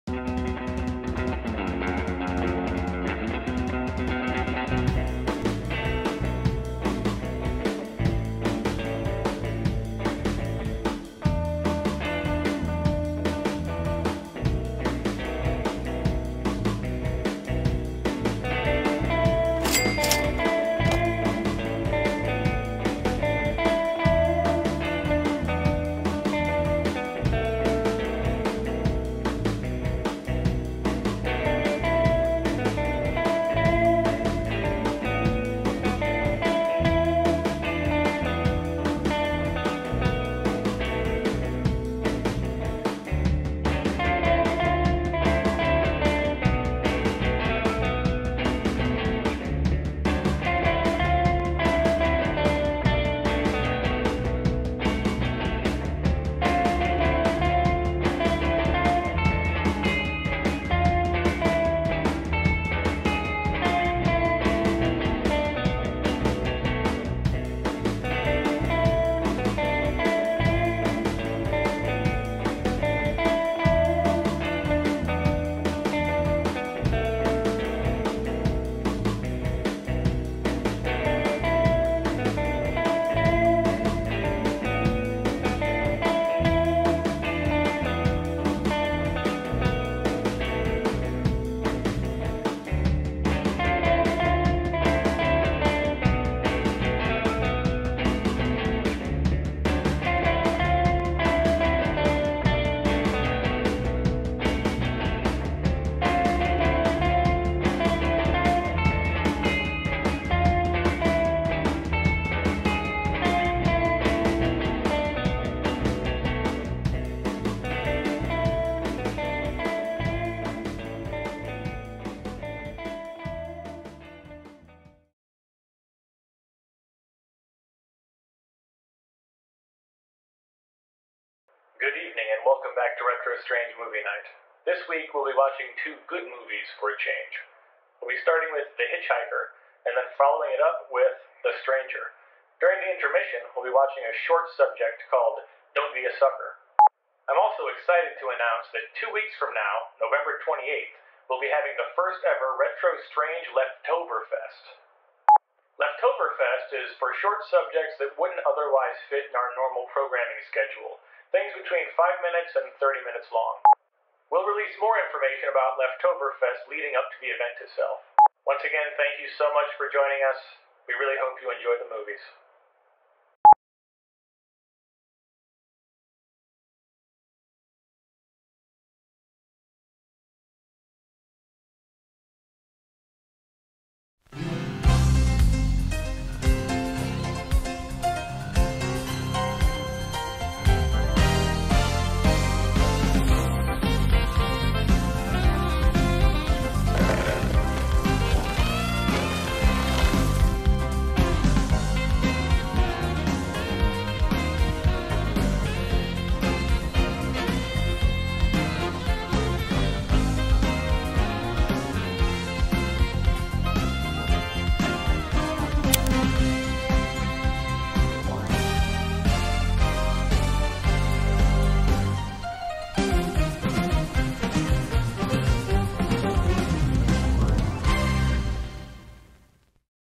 RetroStrange Movie Night #12 Intro
We watched The Hitch-Hiker and The Stranger, and announced Leftoverfest.